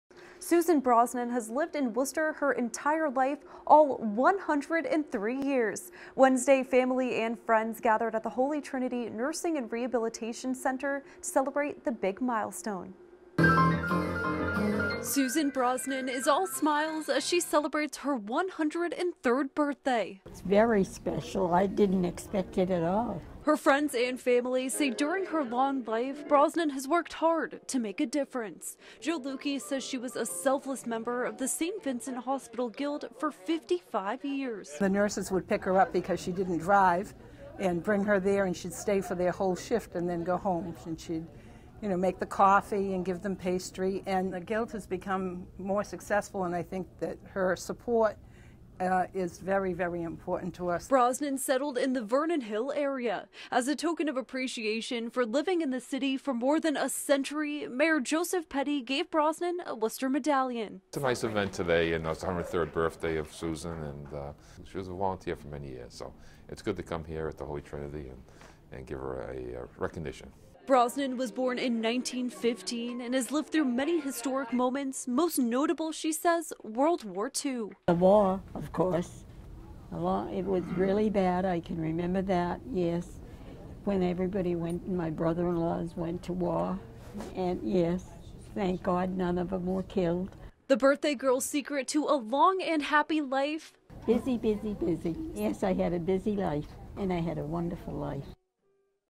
Worcester Woman's 103rd Birthday - December 26th, 2018
Brittany Schaefer reports- Friends and family of Susan Brosan gathered to celebrate her 103rd Birthday.